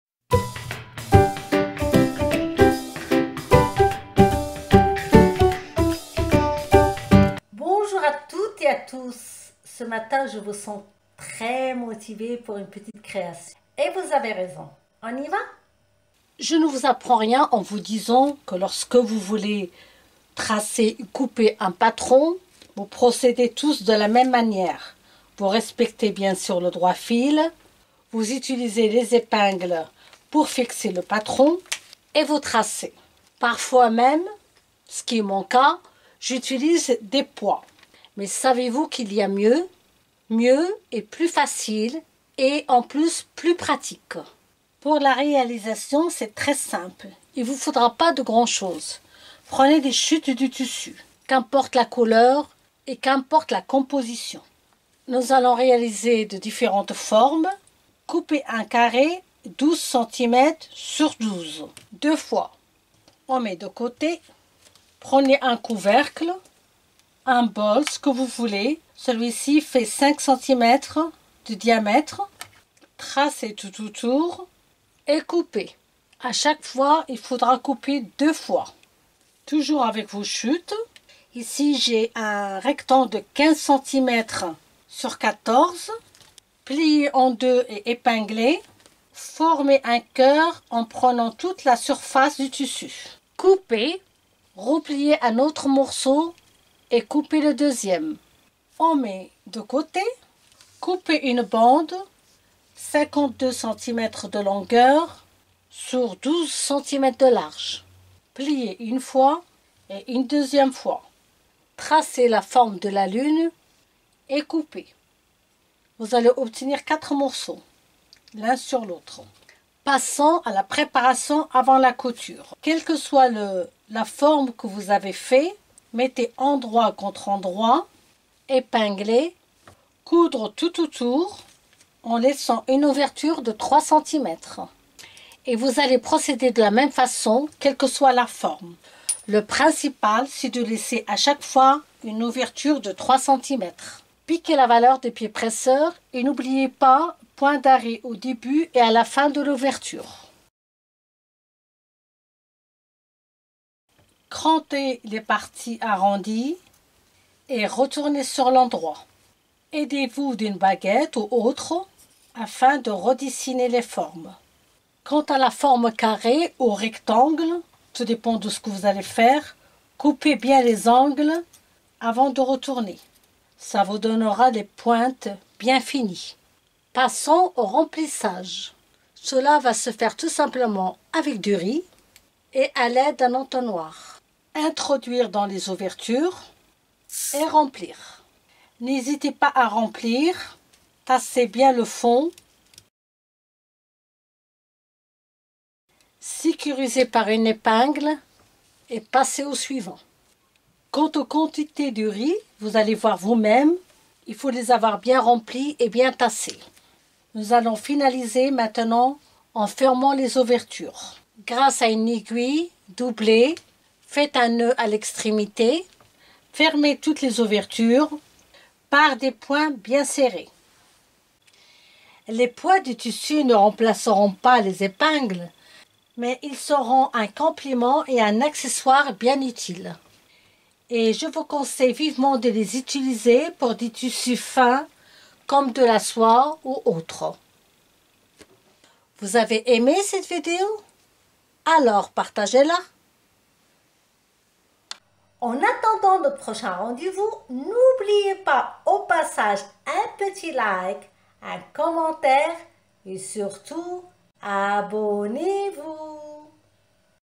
DIY #140 Les poids de couture . 100/100 recyclage . Facile et rapide
Les poids de couture un accessoire idéal pour maintenir les patrons de couture lors de la découpe.
Elles ne remplaceront pas les épingles mais seront  un complément bien utile ( au lieu de boites de conserves ou de pots )
Une belle occasion pour utiliser des chutes de tissus.
Les formes carré , rectangle ,lune ou cœur sont des découpes simples à réaliser .
Le remplissage avec du riz pour aller dans la simplicité mais rien ne vous empêche de le remplacer par le sable ou autres .
Allez lancez -vous et faites moi plaisir partagez la vidéo.
Bonne réalisation à tous .
The sewing weights an ideal accessory to keep the sewing patterns when cutting.
They will not replace the pins but will be a useful complement (instead of cans or jars)
A great opportunity to use scrap fabrics.
The square, rectangle, moon or heart shapes are simple cuts.
The padding with rice to go in simplicity but nothing prevents you from replacing it with sand or other .
Go ahead and do me a favor and share the video.
Good achievement to all .